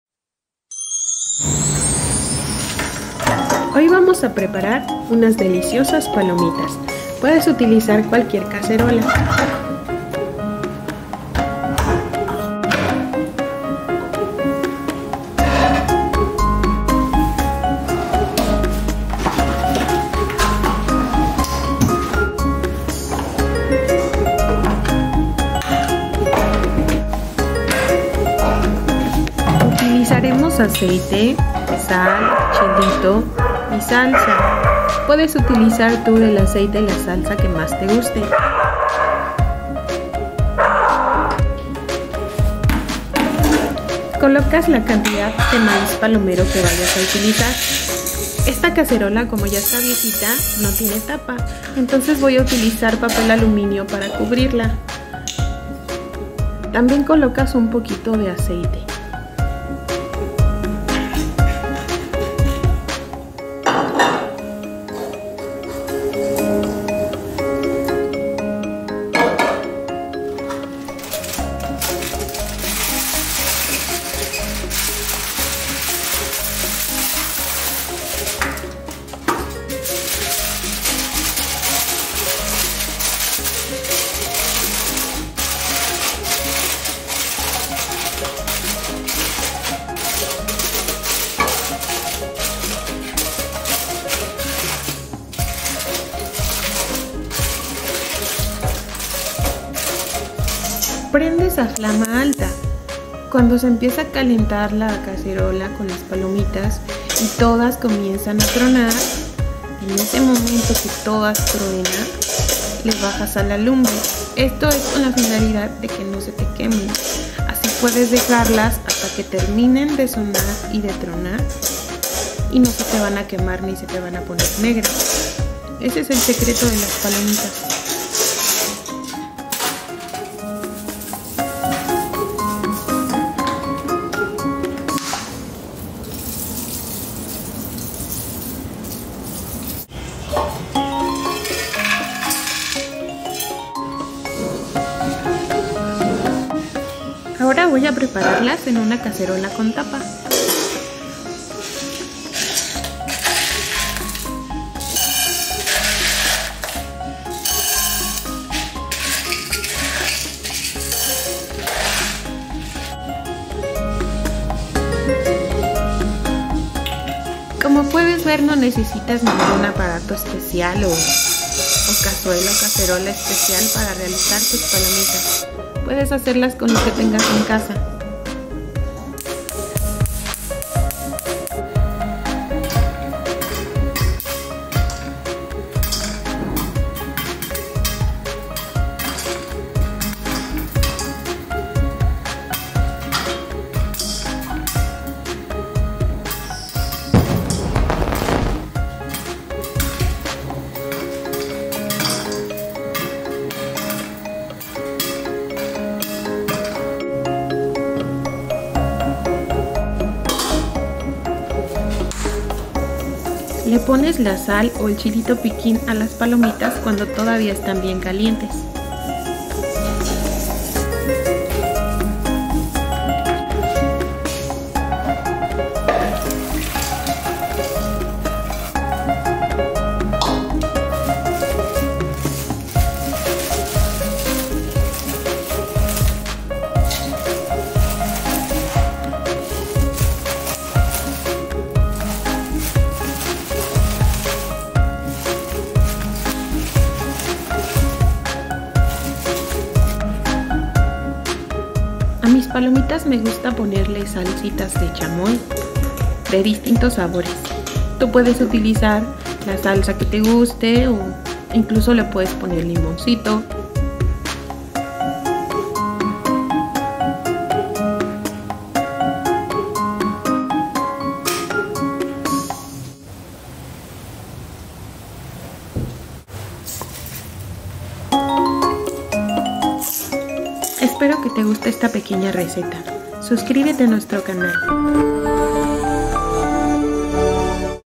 Palomitas de maíz fácil y sin quemarlas. palomitas recetapalomitas popcornrecipe 🍿🌽🤤😄
Prepara unas deliciosas palomitas, para leer, ver tus películas o series favoritas. 🍿🌽
Ingredientes:

Maíz palomero el necesario.
Sal, chile piquín (opcional) al gusto.
Aceite o mantequilla al gusto.
Salsa botanera (valentina, búfalo, chamoy).
Prepare some delicious popcorn, to read, watch your favorite movies or series.

Ingredients:

Popcorn the necessary.
Salt, chili pepper (optional) to taste.
Oil or butter to taste.
Botanera sauce (valentina, buffalo, chamoy).